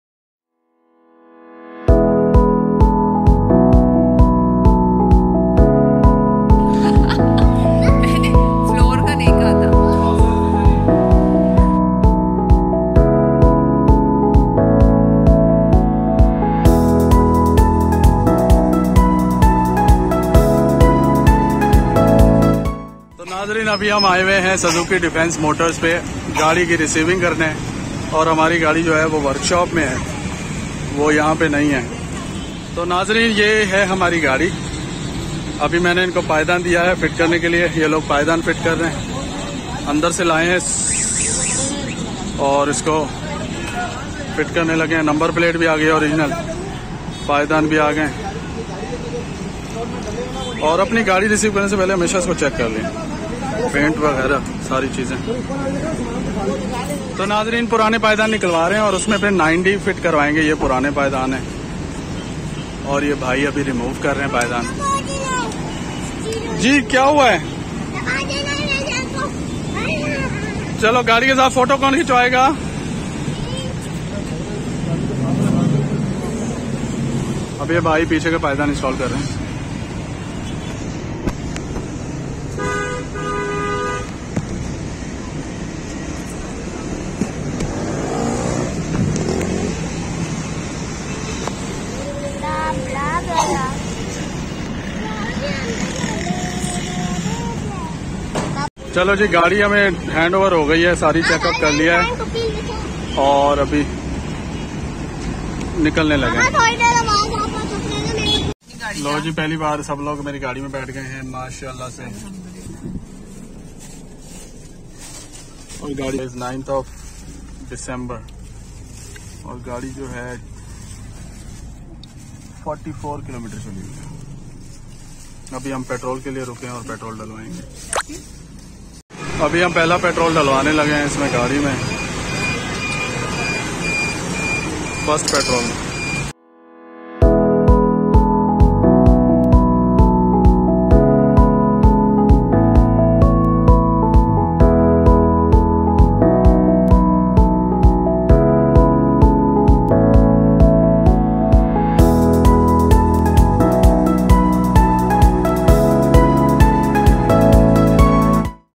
Receiving Delivery of Suzuki Swift GLX CVT from Showroom
Receiving our new car Suzuki Swift GLX CVT Silky Silver from Showroom Suzuki Defense Motors, Karachi.